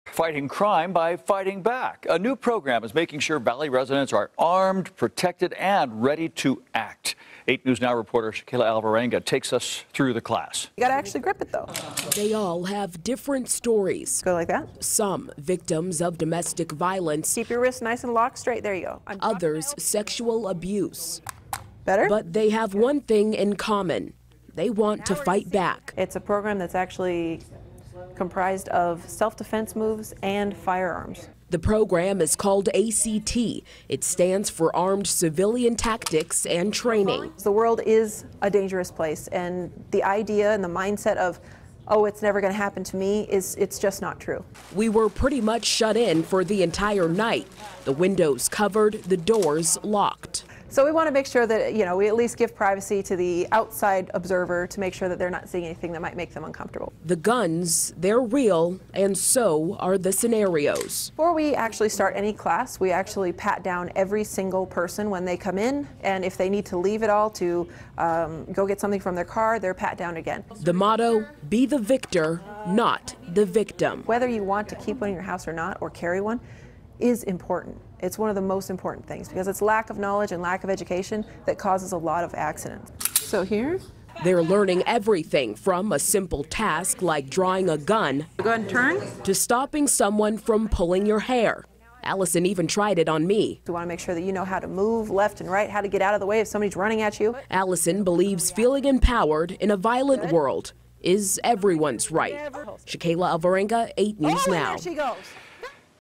Fighting crime by fighting back LasVegasNow KLAS TV Campbell Defense Academy
The News seems to like our program. COME see for yourself why we are a 5 Star Rated Guaranteed Firearms/Self-Defense program.